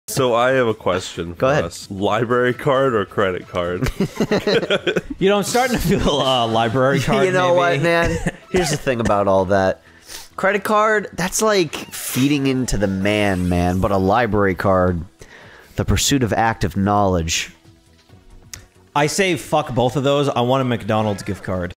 WHAT WOULD YOU PICK?
Library Card or Credit card? Or mcdonalds gift card? A tough question indeed